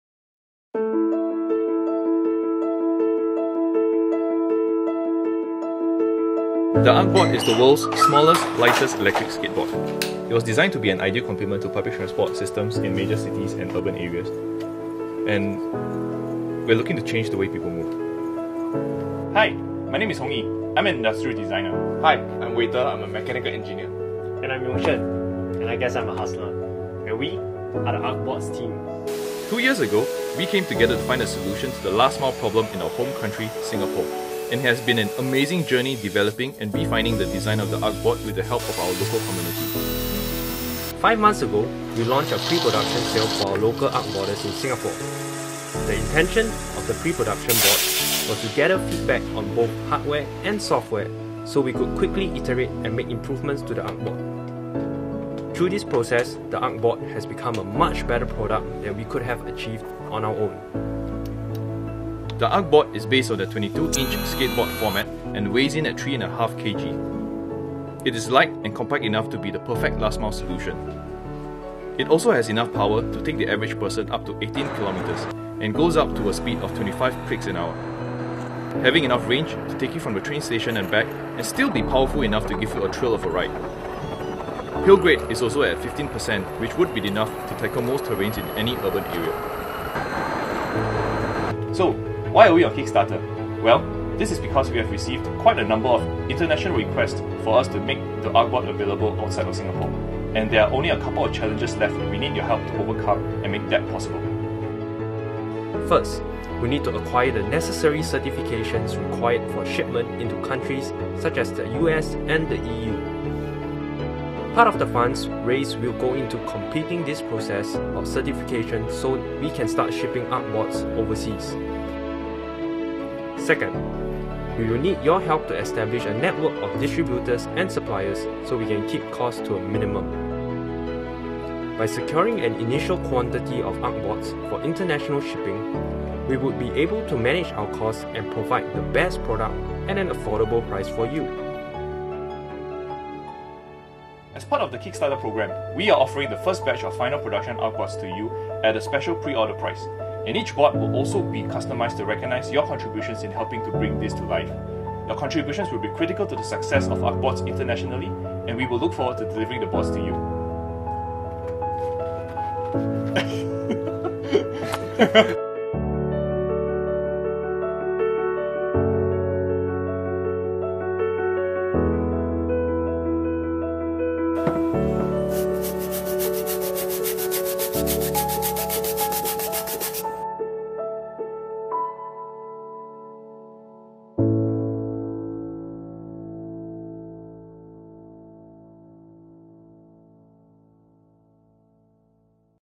World's Smallest Lightest Electric Skateboard on Kickstarter!
World's Smallest Lightest Electric Skateboard on Kickstarter! We're live on Kickstarter!

After almost two years of design, development, iteration and refinement, the Arc Boards Team is excited to share that we have officially launched our Kickstarter campaign!

The Arc Board is the World's Smallest, Lightest Electric Skateboard, and has a range of 18km and speeds up to 25km/h. Weighing just 3.5kg it is also the most portable electric vehicle available.

The new 121C Arc Aileron - "Pure Carbon Electric Skateboard", now available on Kickstarter

The Original Arcboard - "The smallest, lightest, last mile solution"